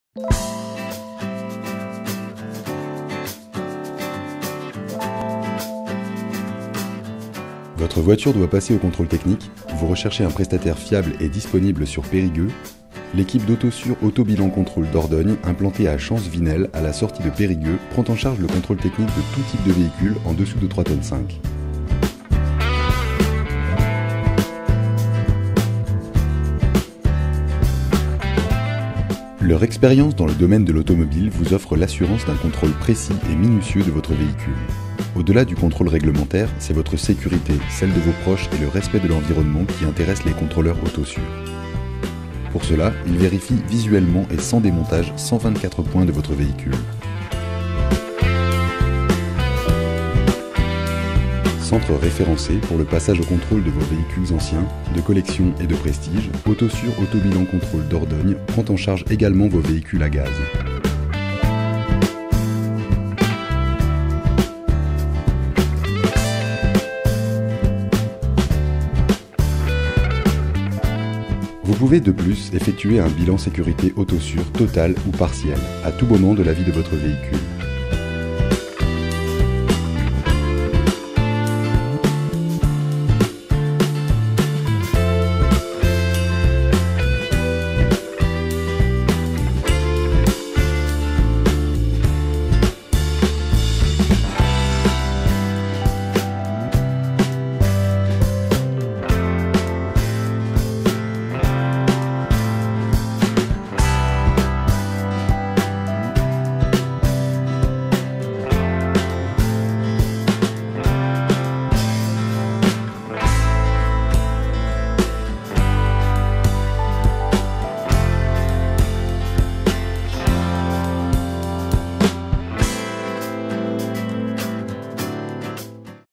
Contrôle technique Périgueux - Auto bilan contrôle Dordogne (24)
Votre voiture doit passer au contrôle technique ? Vous recherchez un prestataire fiable et disponible sur Périgueux ?
L'équipe d'Autosur auto bilan contrôle Dordogne, implantée à Champcevinel, à la sortie de Périgueux, prend en charge le contrôle technique de tout types de véhicules en dessous de 3,5 tonnes.

Leurs expérience dans le domaine de l'automobile vous offre l'assurance d'un contrôle précis et minutieux de votre véhicule. Au-delà du contrôle réglementaire, c'est votre sécurité, celle de vos proches et le respect de l'environnement qui intéressent les contrôleurs Autosur. Pour cela, ils vérifient visuellement et sans démontage 124 points de votre véhicule.

Centre référencé, pour le passage au contrôle de vos véhicule anciens, de collection et de prestige, Autosur auto bilan contrôle Dordogne prend en charge également vos véhicules à gaz.

Vous pouvez de plus, effectuer un bilan sécurité Autosur, total ou partiel, à tout moment de la vie de votre véhicule.
A bientôt !